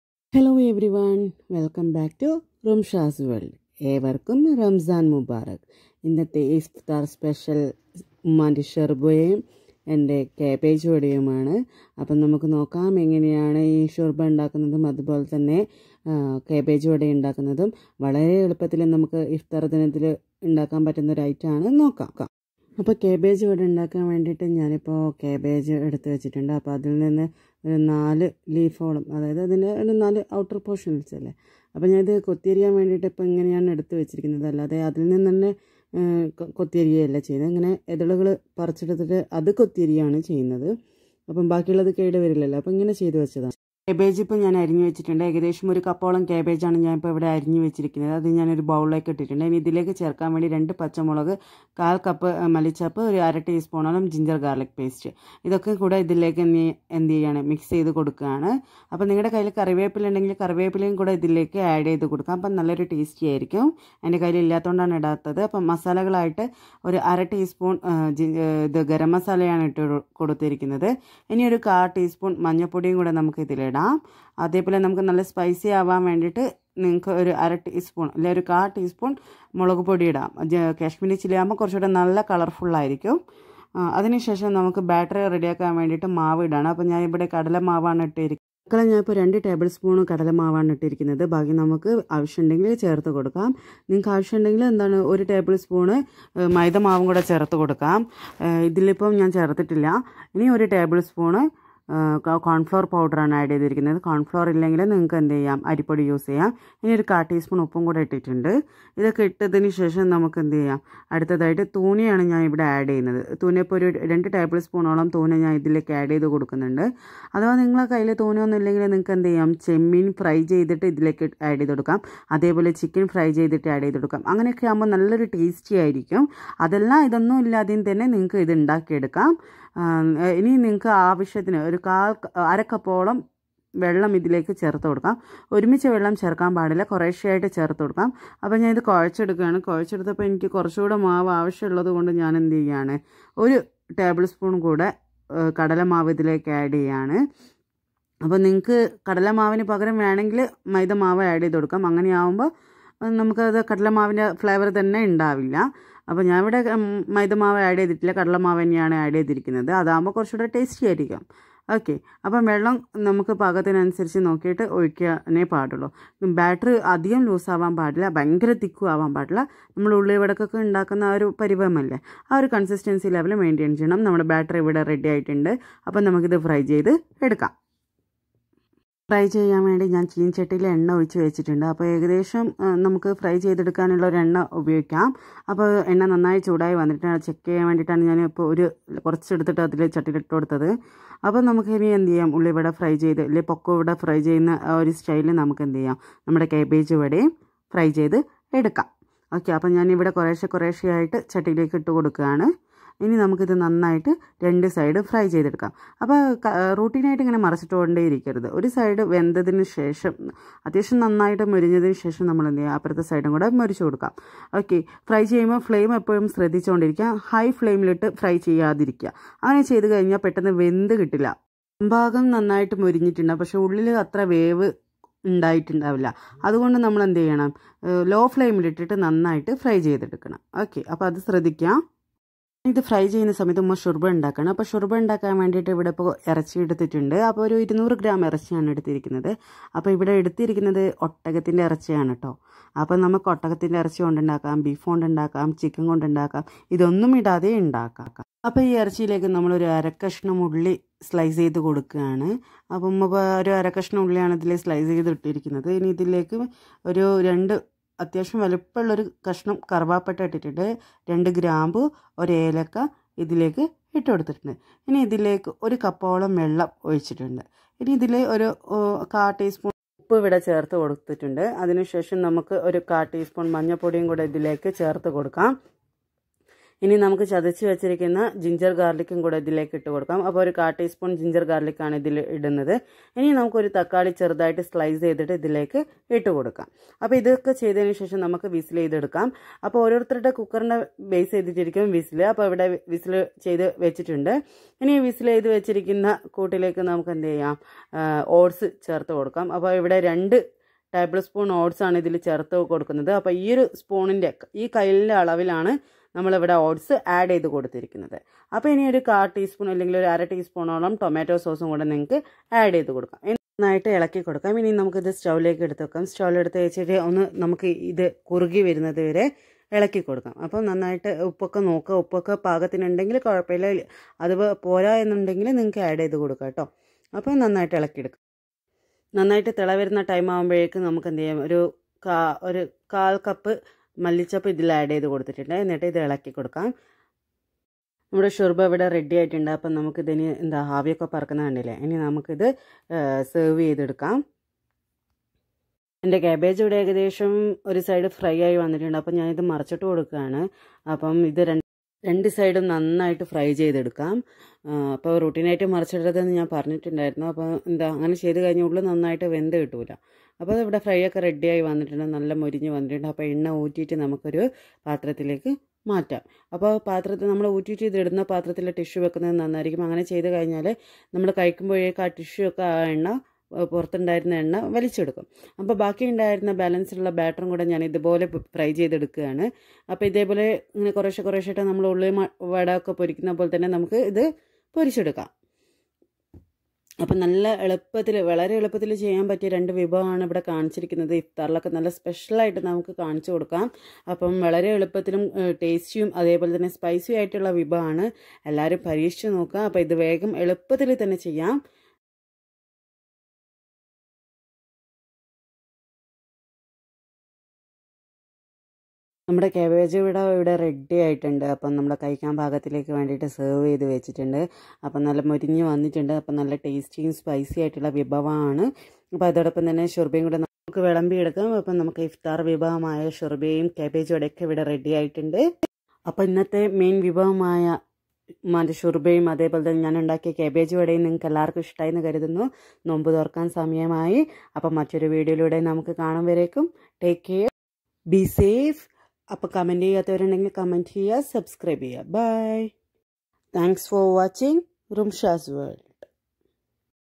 ഉമ്മാന്റെ ഒട്ടക ഷുർബയും എന്റെ കാബേജ് വടയും.
Ingredients:
Cabbage Vada
1. Cabbage -1 Cup
2.Chillies -2
3. Coriander leaves-1/2 Cup
4. Ginger Garlic paste - 1/2 teaspoon
5.Garam Masala - 1/2 teaspoon
6. Turmeric powder- 1/4 teaspoon
7. Groundnut flour- 3 tablespoon
8. Salt-1/4 teaspoon
9.Tuna - 2 table spoon
10.Water - 1/2 Cup
11. Oil - 1 Cup

Shurba:
1. Meat ( Camel) - 200 gm
2. Onion - 1/2 sliced
3. Turmeric powder- 1/4 ts
4. Cinnamon - 2 pieces
5. Grambu - 2 pieces
6. Cardamom- 1 piece
7. Salt - 1/4 ts
8. Water - 1cup
9. Oats - 2 tb
10. Coriander leaves- 1/4 Cup

Be safe Be Home

Ramadan Mubarak
#  Rumsha's World#